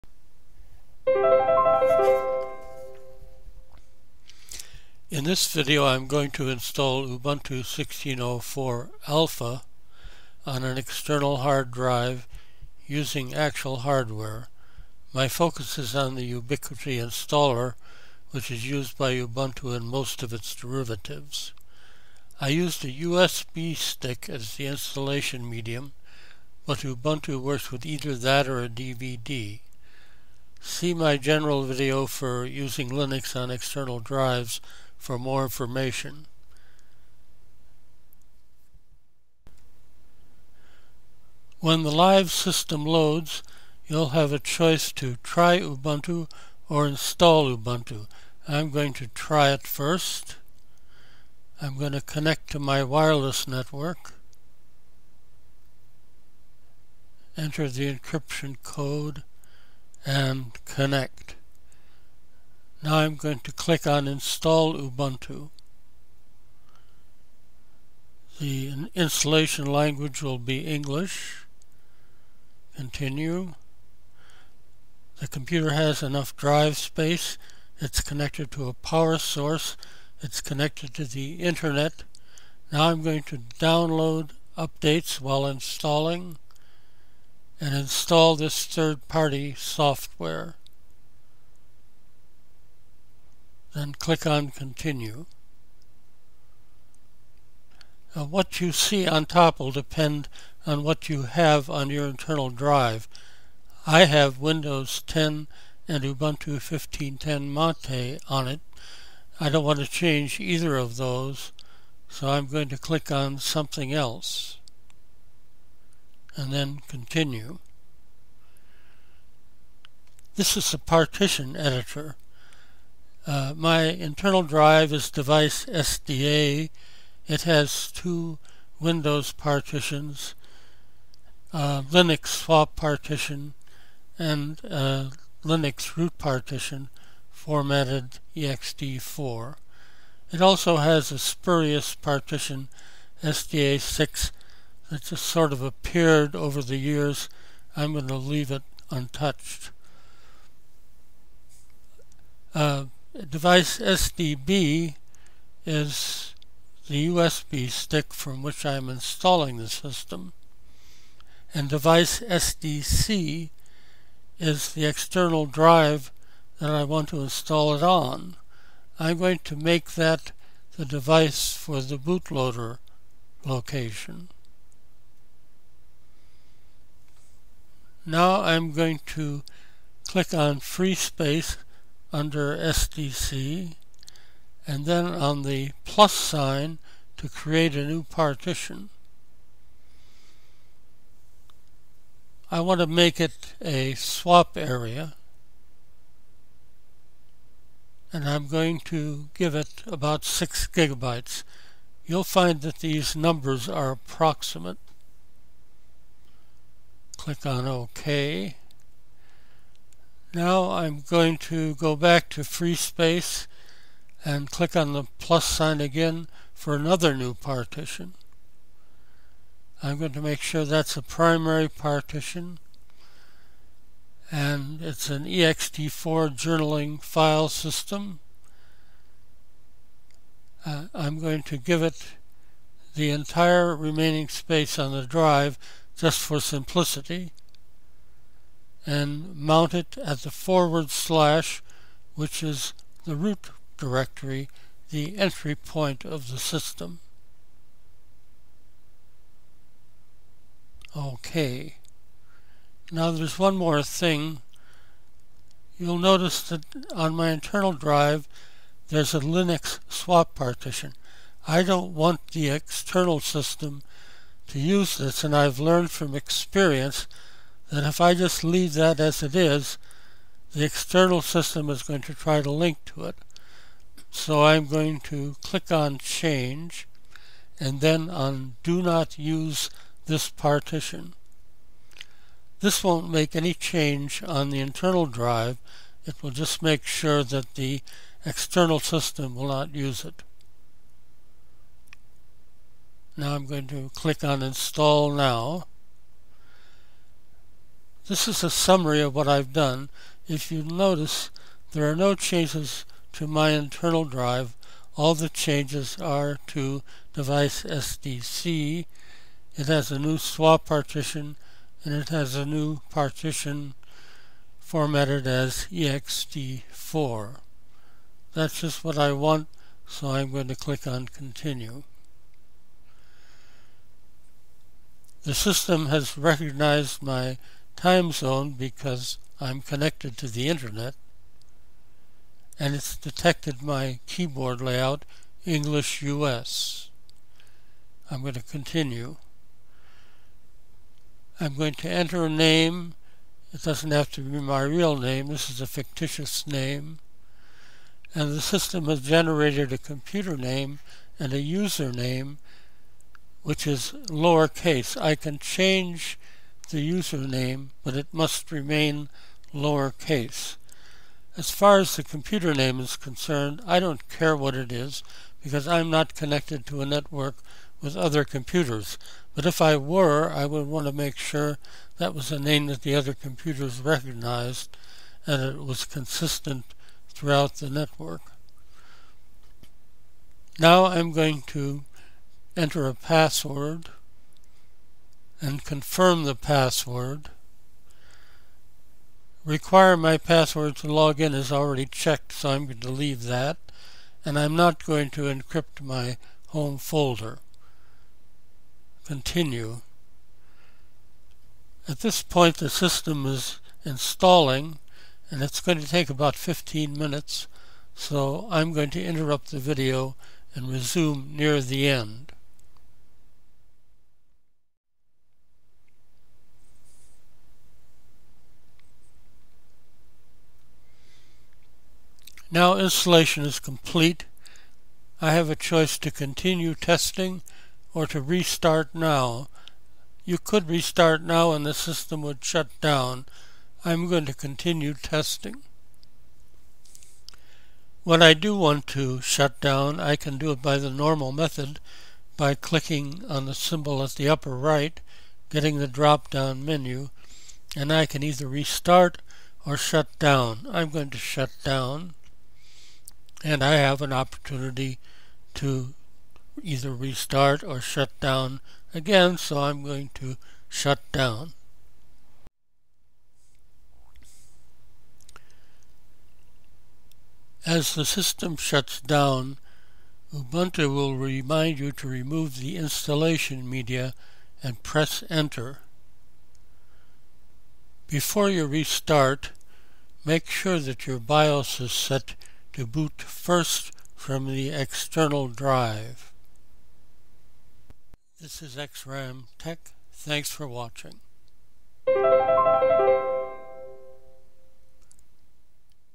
Install Ubuntu 16.04 on an external drive
Step by step instructions for installing Ubuntu on an external drive.